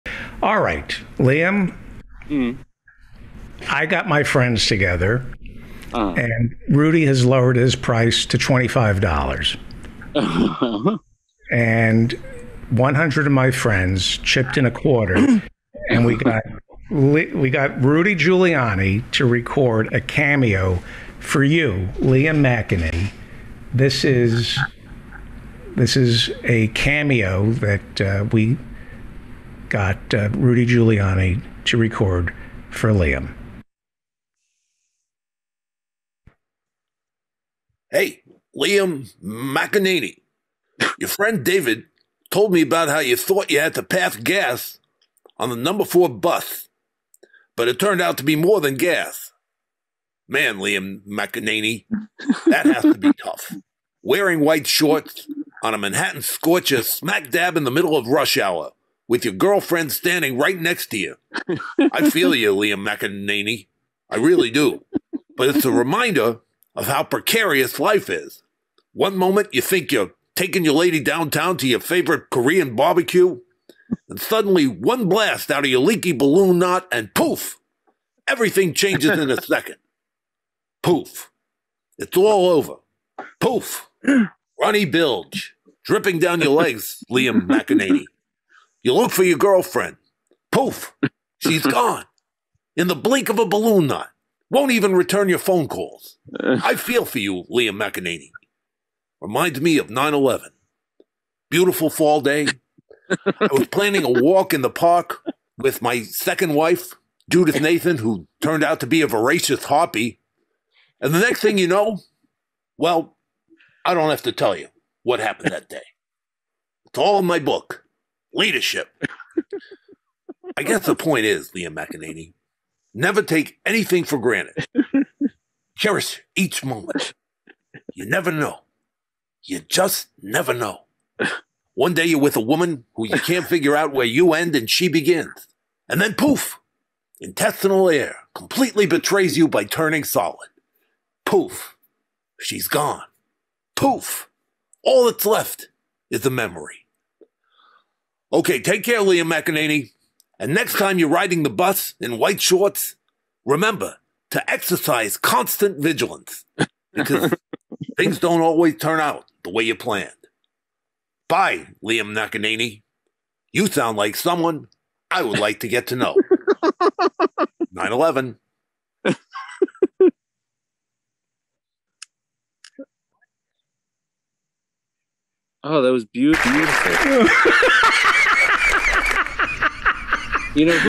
Rudy Giuliani's Cameo For Our Friend Who's Been Down In The Dumps
Our good friend Liam has been feeling kind of down lately. So Robert Smigel and I got some friends to chip in and buy a personalized message from Rudy Giuliani on Cameo. It's amazing how a few kind words can lift a man up. Thank you America's Mayor.
We livestream here on YouTube every Monday and Thursday starting at 5:00 PM Eastern and go until 11:00 PM. Please join us!